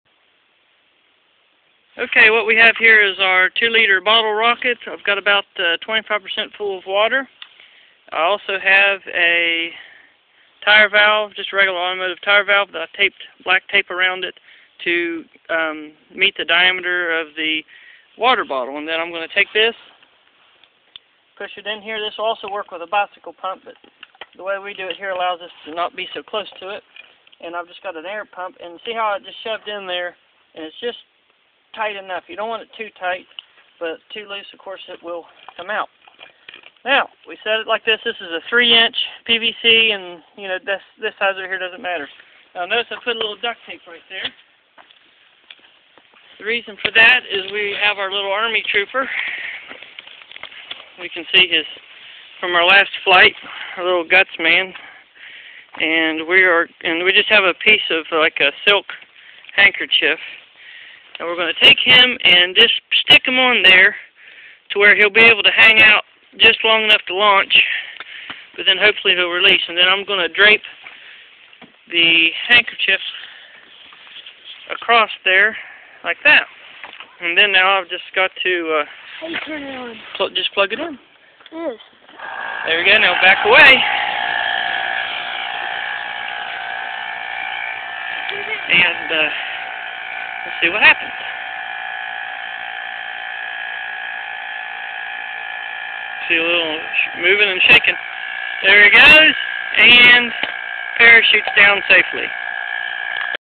2 liter Water Bottle Rocket Launch with army guy silk Parachute Air Pressure Bowie
2 liter Water Bottle Rocket Launch with army guy silk Parachute Air Pressure Bowie